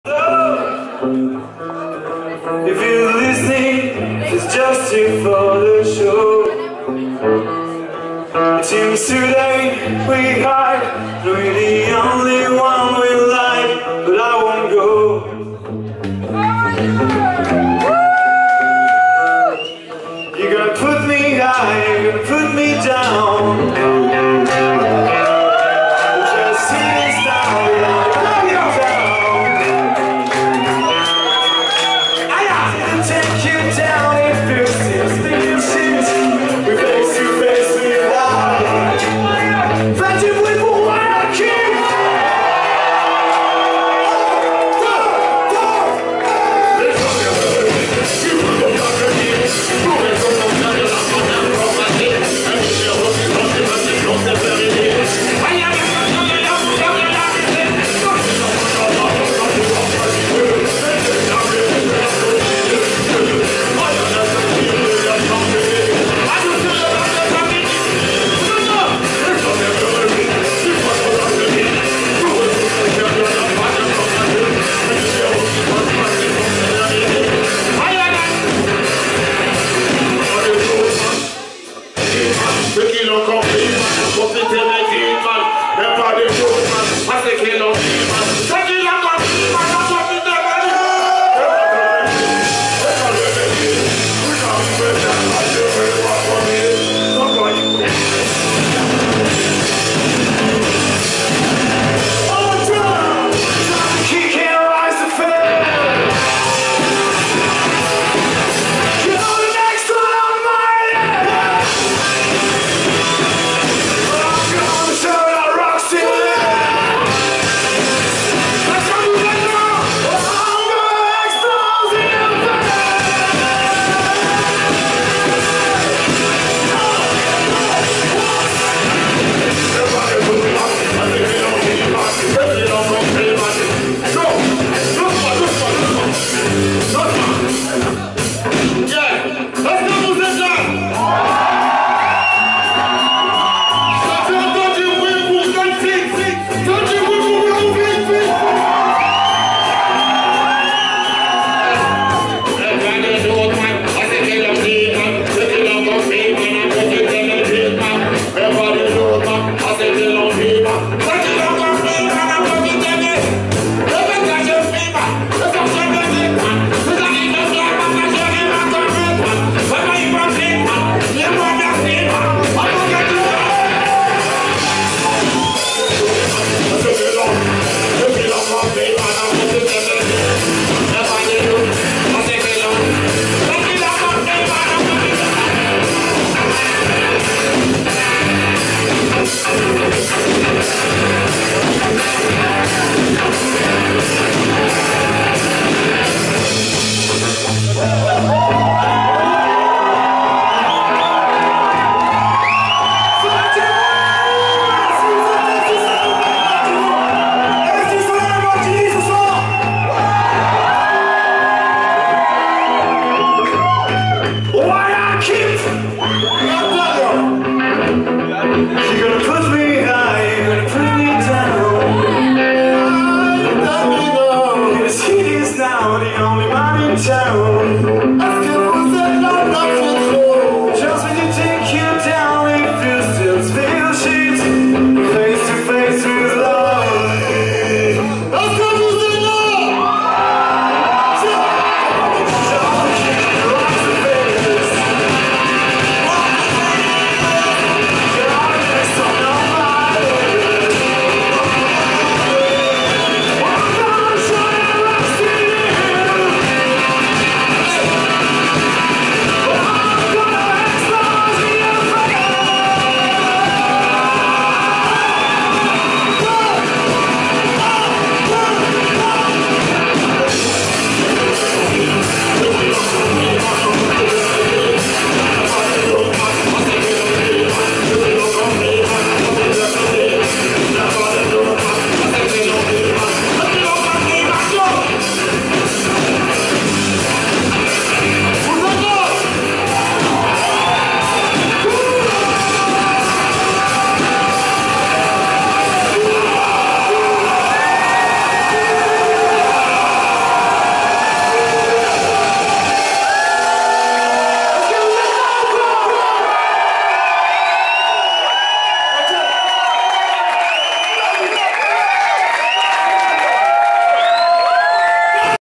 Blackfeet Revolution feat. Royal Keep (Live @ La Maroquinerie)
La Maroquinerie, Paris - Le 10/10/2014